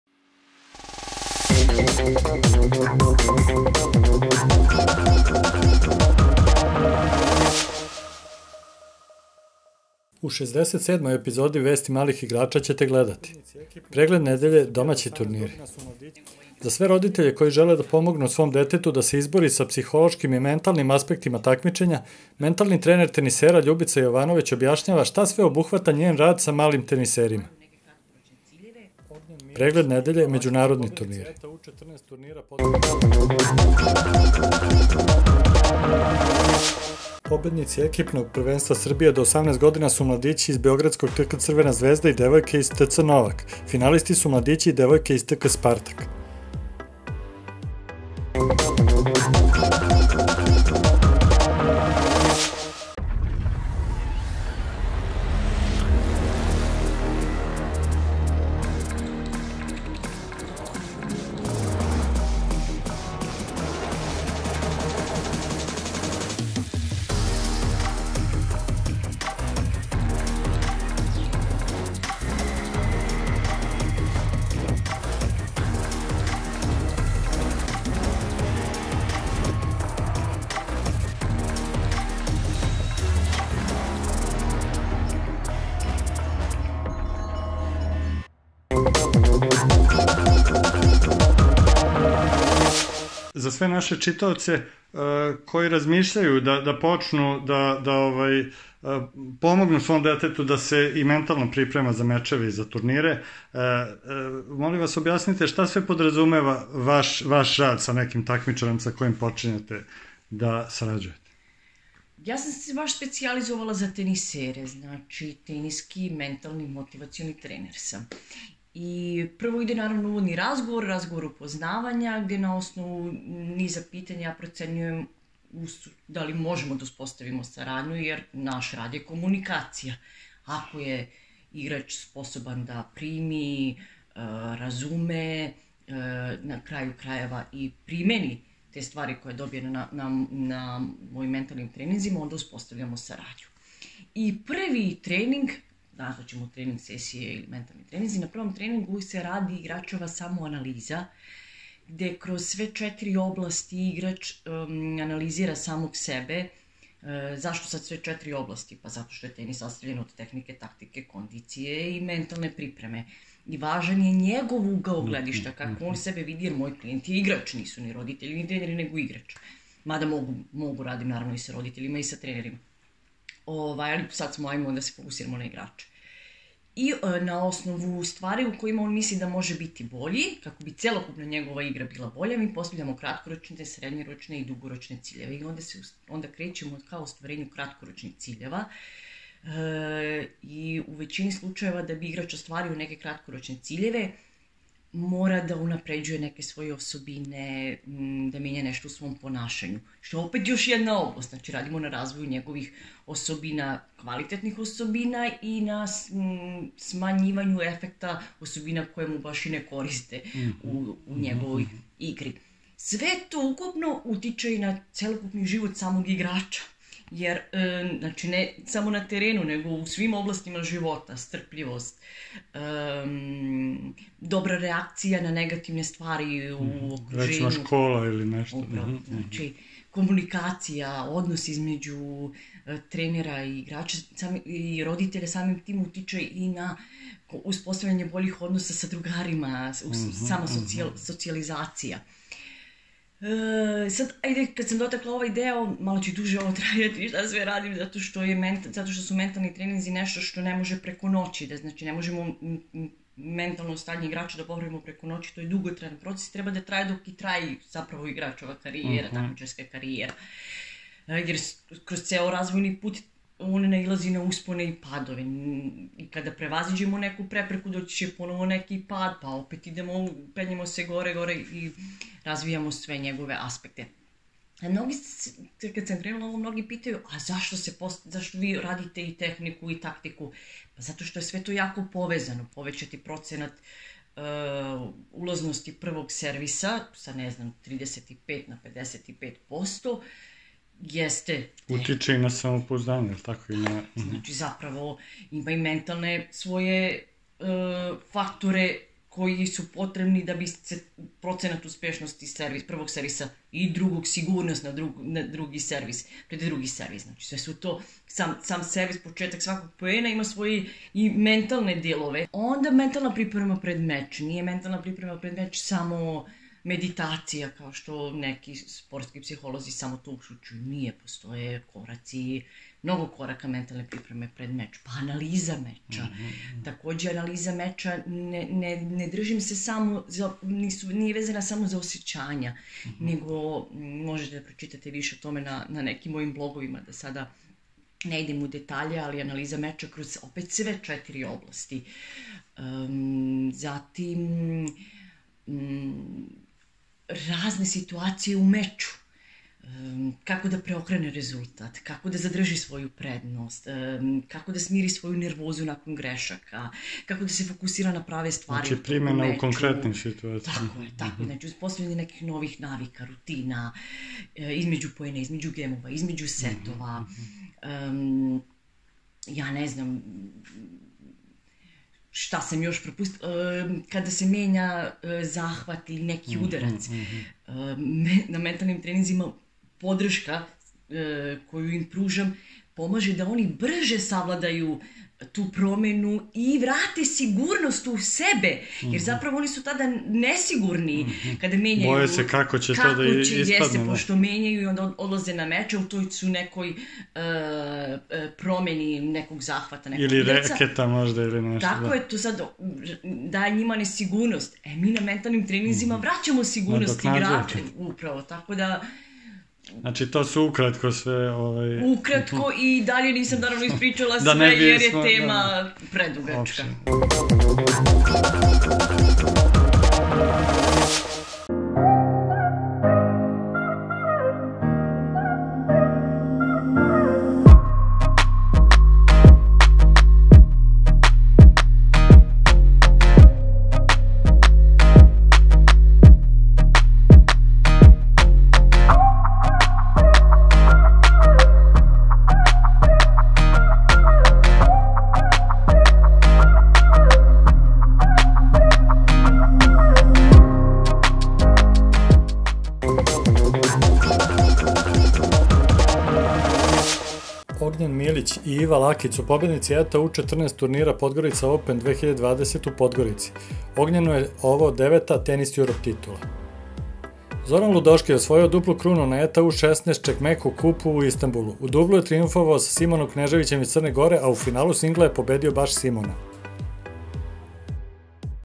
Vesti Malih igrača, 67. epizoda, 29.11.2020.
У шездесет седмој епизоди „Вести Малих играча“ ћете гледати:
0:09 Најава 67. епизоде
0:32 Преглед недеље: домаћи турнири
1:29 За све родитеље који желе да помогну свом детету да се избори са психолошким и менталним аспектима такмичења, ментални тренер тенисера Љубица Јовановић објашњава шта све обухвата њен рад са малим тенисерима
7:32 Преглед недеље: међународни турнири